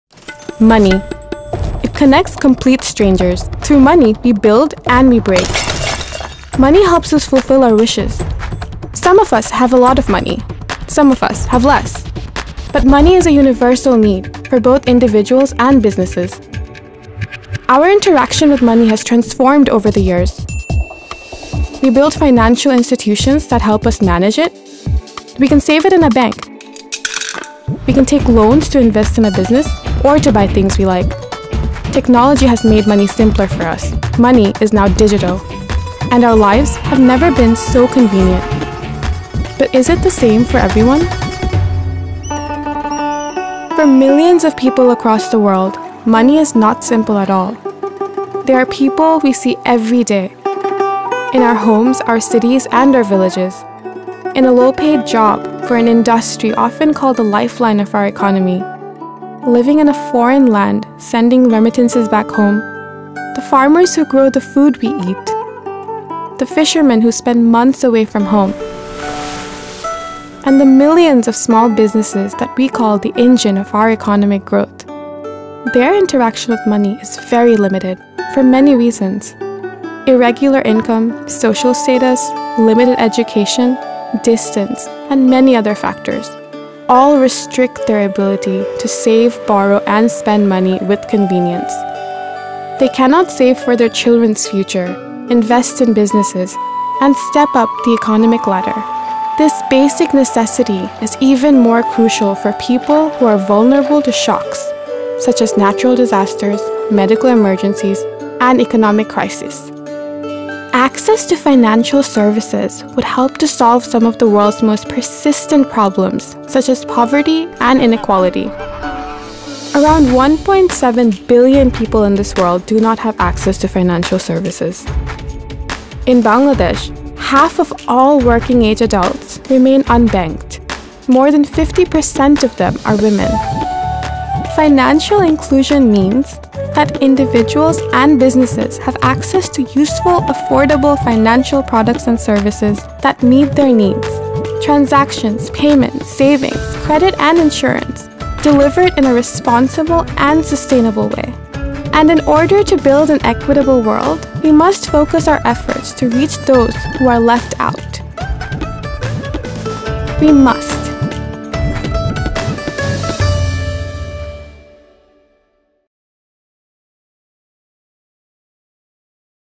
What is Financial Inclusion? | Short Film | BRAC
Around 1.7 billion people in this world do not have access to financial services. In Bangladesh, half of all working-age adults remain unbanked. More than 50% of them are women.

Access to financial services would help to solve some of the world’s most persistent problems such as poverty and inequality. And in order to build an equitable world, we must focus our efforts to reach those who are left out.